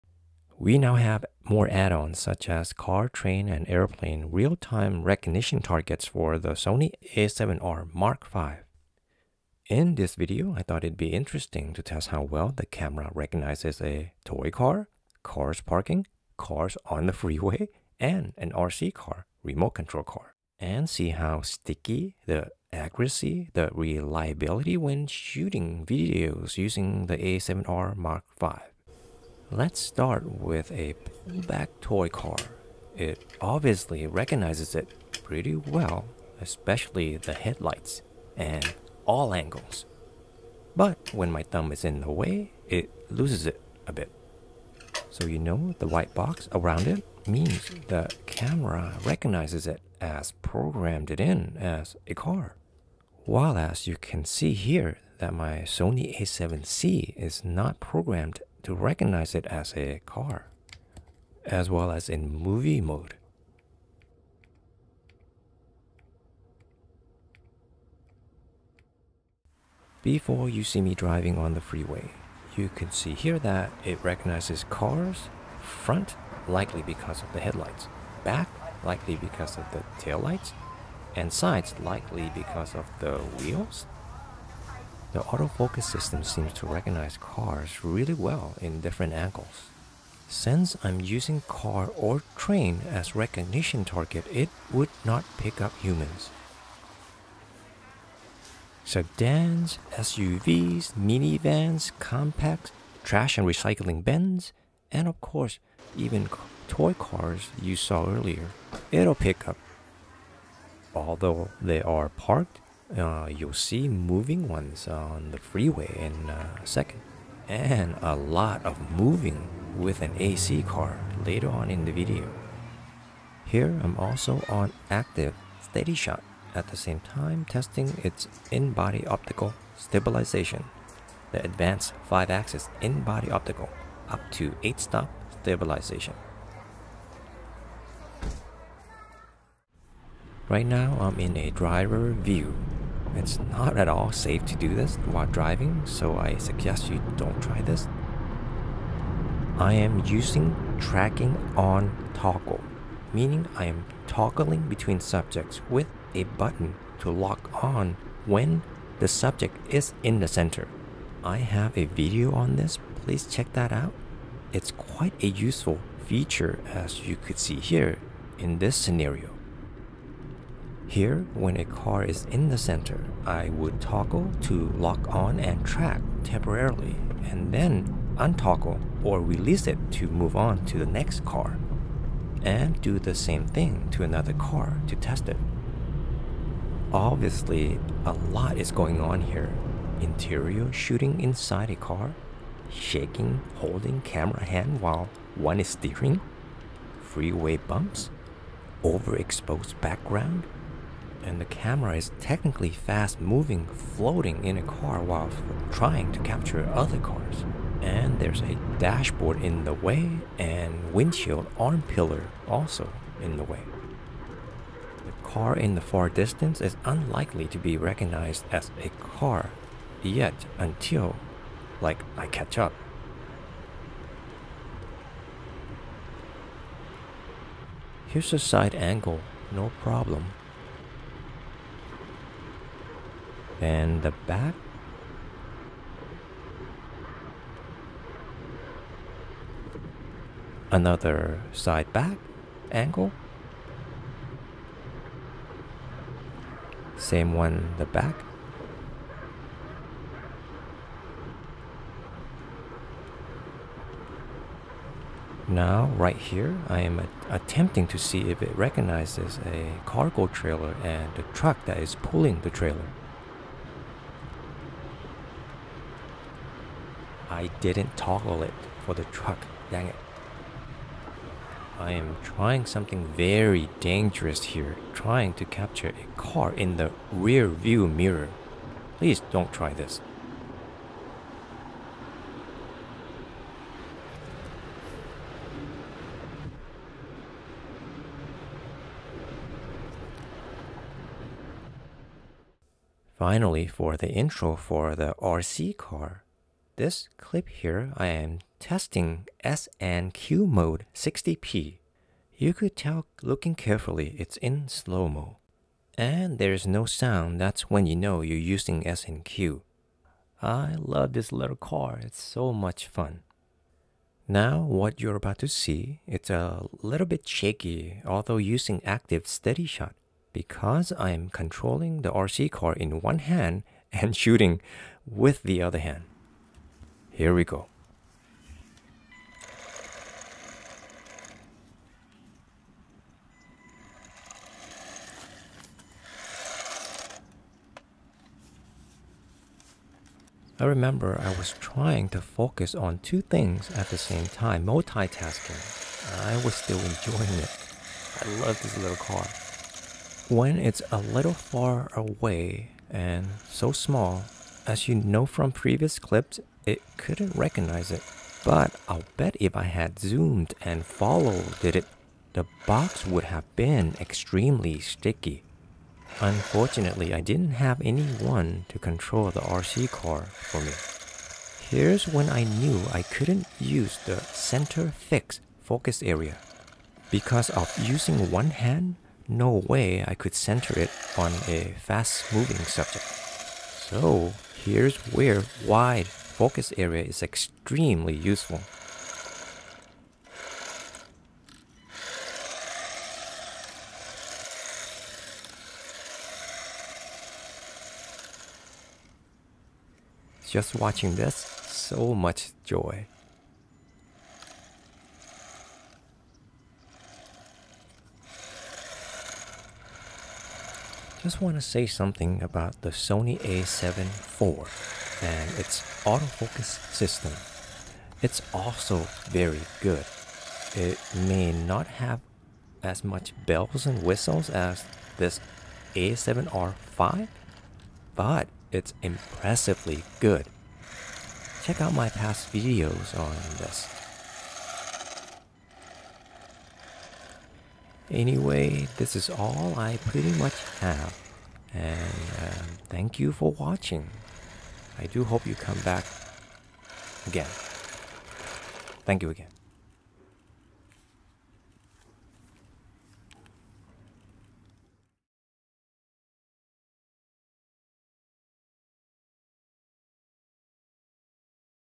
Sony A7R V Video Real-Time AutoFocus: CARS
You know we have always had the several Focus Modes:
Single-shot AF (AF-S), Automatic AF (AF-A), Continuous AF (AF-C), DMF (manually focusing after auto focusing), and of course Manual Focus (MF).

PLUS With all the Focus Areas: Wide, Zone, Center Fix, Spot, Expand Spot + (plus) Tracking

And on top of all that we have had human, animal, and even bird recognition on previous models.

We now have more add-ons such: Car, Train, and Airplane  Real-Time recognition Targets for the Sony A7R mark V.

In this video, I’d thought I would be interesting to test how well the camera recognizes a toy Car, CARS parking, CARS on the Freeway and an RC car (Remote Control car). And see how sticky, the accuracy, and the reliability when shooting Videos using the A7R V.

I also show the difference between the Sony A7R V and Sony A7C when they focus on a toy car.

Here I’m also on Active SteadyShot. At the same time testing its in-body optical stabilization. The Advanced 5-axis in-body optical (up to 8 stops) stabilization. In addition, I also test S&Q mode 60p for slow-mo.

Moreover, I also use Tracking On (Toggle), a hidden feature on the camera.